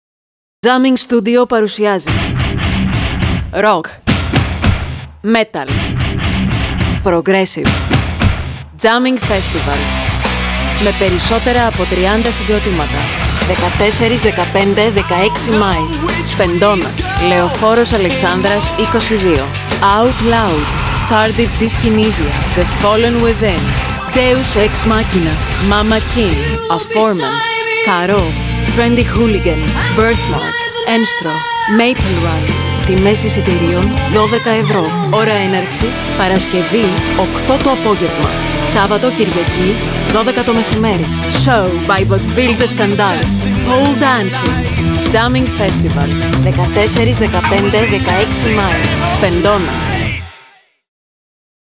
JAMMIN.G MUSIC PUNCH.avi
To Jammin.G  Studio και οι συνεργάτες του διοργανώνουν ένα τριήμερο      μουσικό φεστιβάλ στην αθηναική μουσική σκηνή ΣΦΕΝΤΟΝΑ, Παρασκευή,
Σάββατο, Κυριακή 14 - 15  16  Μαίου, με 36 !!!  μουσικά σχήματα  και 12ωρη διάρκεια ανά ημέρα. (PROGRESSIVE DAY - METAL DAY  ROCK DAY).
     Ο χαρακτήρας του φεστιβάλ είναι νεανικός (18-102)  και θα συμμετέχουν ανερχόμενα και ήδη αγαπημένα γκρουπ της ελληνικής rock & metal σκηνής με τη δική τους ξεχωριστή δυναμική στο χώρο. Η παραγωγή θα αναλάβει τα σκηνικά, τη βιντεοσκόπηση, την ηχογράφηση, την τοποθέτηση και τεχνική υποστήριξη των video projectors. Επιπλέον θα διεξαχθούν side events με την παρουσία του μάγου King Robert και χορευτριών από το pole dancing lessons , που μαζί με τις Vaudeville  de Scandale θα απογειώσουν το τριήμερο !!!
     Στόχος αυτού του  πρωτοποριακού φεστιβάλ είναι η ανάδειξη και ενίσχυση  της ελληνικής underground rock και metal σκηνής στα εγχώρια μουσικά δρώμενα !
Η ελληνική μουσική σκηνή έχει παρόν και μέλλον και σας προσκαλεί να τη στηρίξετε ενεργά με την παρουσία σας!
    Τέλος, παραθέτουμε το οριστικό line-up των συγκροτημάτων που θα συμμετέχουν στο JAMMIN.G FESTIVAL 2010 .

Παρασκευή,  ώρα  έναρξης   20:00
PROGRESSIVE ROCK DAY 14th May 2010

Birthmark
Karro
Enstro
Maplerun
Monkey Business

+ Live dj Set by Nicholas Dales
warm up Dj Gova

Σάββατο
METAL DAY 15th May 2010

Outloud
Tardive Dyskinesia
The Fallen Within
Crucifier
Prejudice Reborn
Trendy Hooliguns
Godfather
Postraumatic
Echthos
Chaos Reign
Winterflame
Social Rejection
My Colors Dying
Spirits Engaged
Dark Elegy

Κυριακή
ROCK DAY 16th May 2010

Deus Ex Machina
Mamma Kin
Τhe Raindogs
Afformance
Blocking Minority
Spank 16
The Lizardz
Conium
Aim.Shoot. Kill
Middle Fingers High
Ξεχασμένοι Ήρωες
Under Construction
Lie
Even More
Jetlag
4 Guns

Με  εκτίμηση ,

ΓΙΩΡΓΟΣ ΟΥΖΟΥΝΙΔΗΣ & συνεργάτες
Διεύθυνση  παραγωγής  JAMMIN.G STUDIOS / JAMMIN.G FESTIVAL